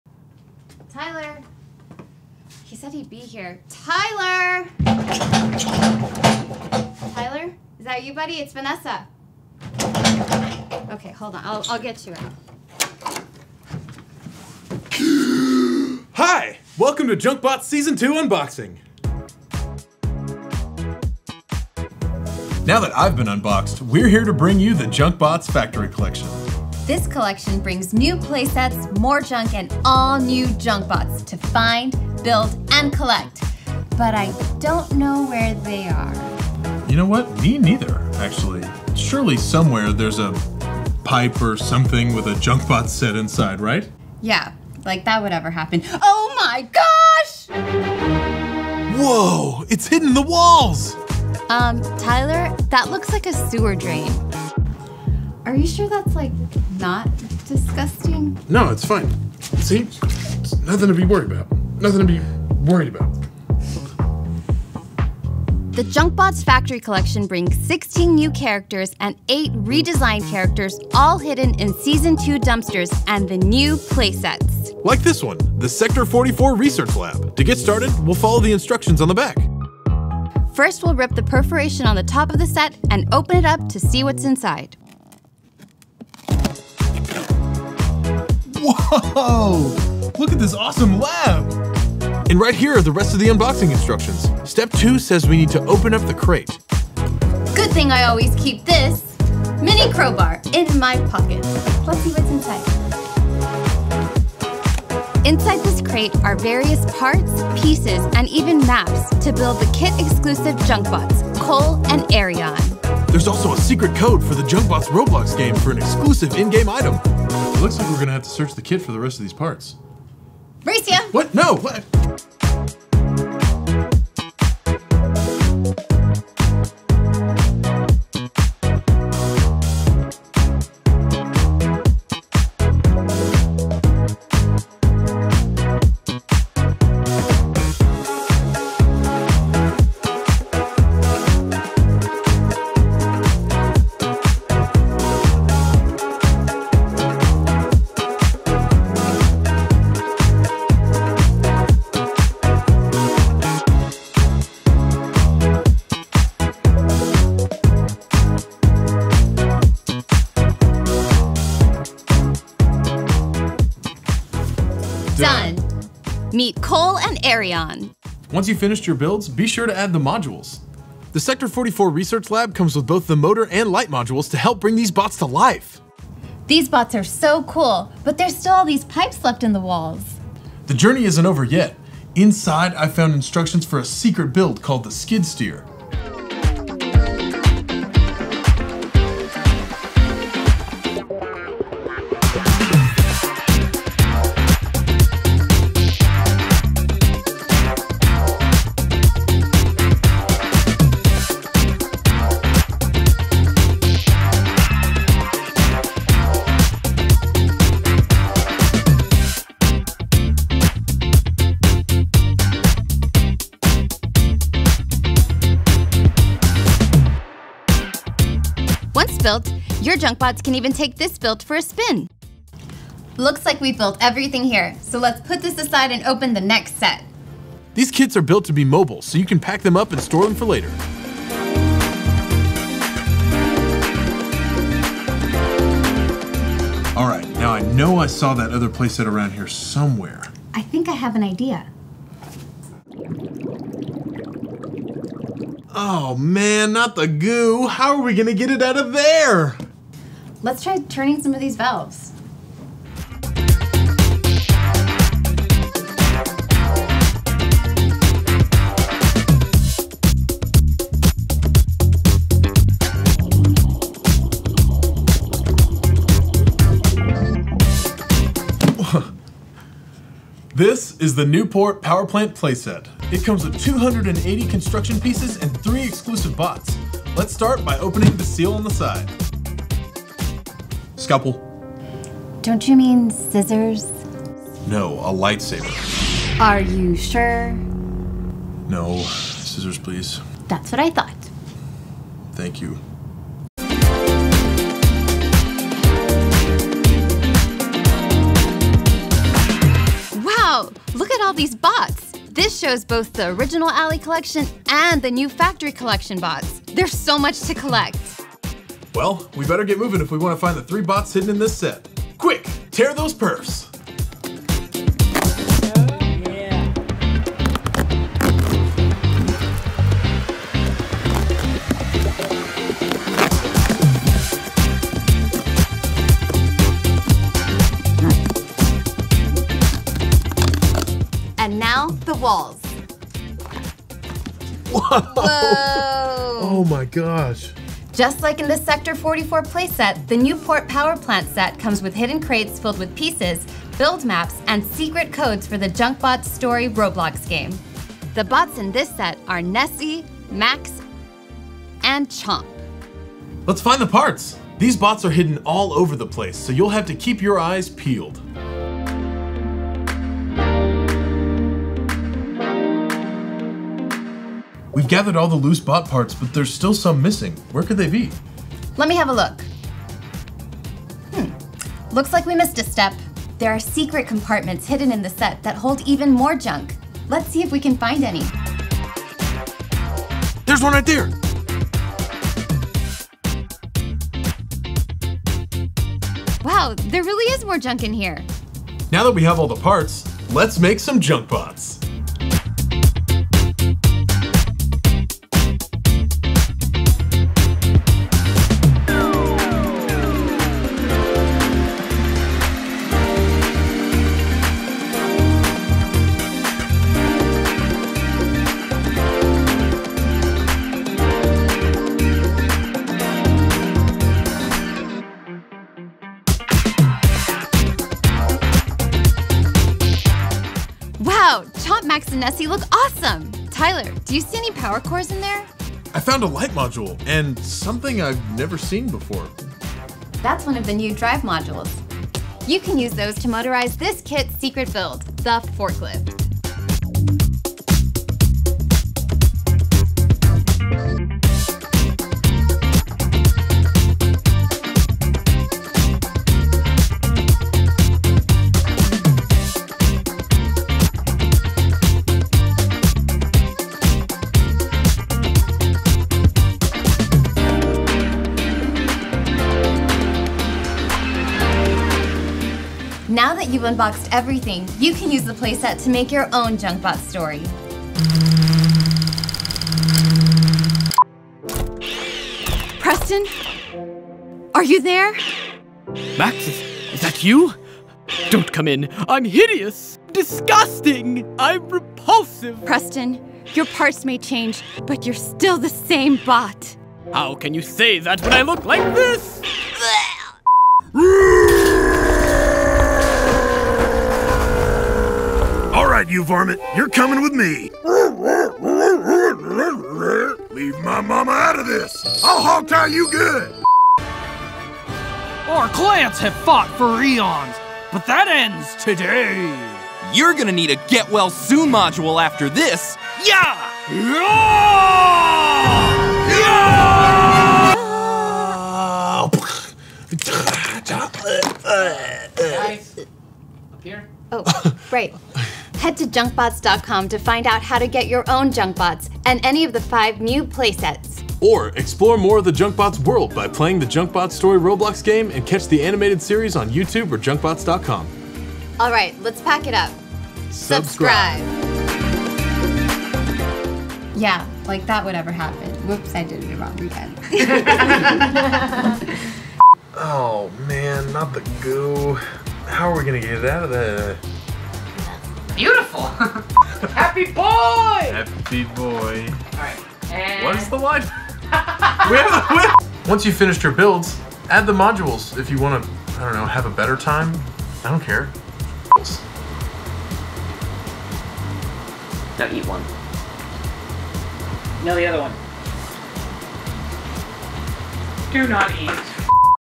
Unboxing your JUNKBOTS Season 2!
JUNKBOTS are back with all-new playsets, secret builds, and new bots to find and build!